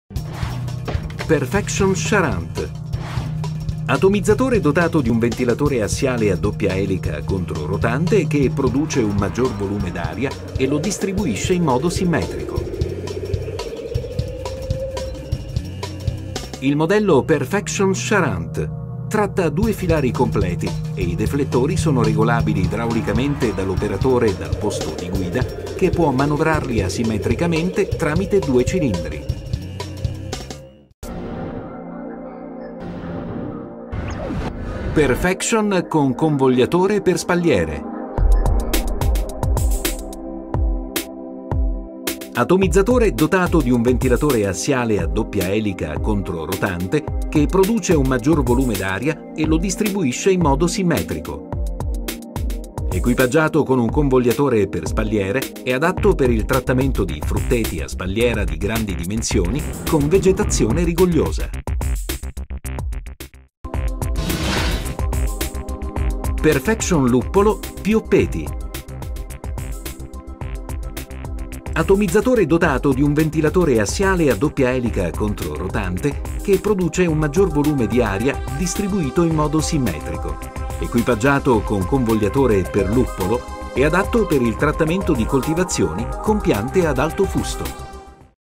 PERFECTION Ideal srl ITALIA - ATOMIZZATORE PER FRUTTETO, VIGNETO, AGRUMETI, PIANTE ALTE
Perfection é un atomizzatore con ventilatore assiale a doppia elica controrotante: una prima elica con rotazione destrorsa e una seconda con rotazione sinistrorsa. La controrotazione elimina l’effetto elicoidale  onsentendo una distribuzione dell’aria perfettamente simmetrica da ambo i lati. Le due eliche, inoltre, producono un volume d’aria assai elevato che permette di realizzare trattamenti ottimali anche in coltivazioni di ampie dimensioni e con vegetazione rigogliosa. Non solo, grazie all’applicazione di specifici convogliatori, è possibile trattare coltivazioni con diversi sesti d’impianto e ottenere una distribuzione del prodotto ottimale e mirata sull’obiettivo. È disponibile nelle versioni trainate con cisterne da 800, 1000, 1500, 2000 e 3000 litri.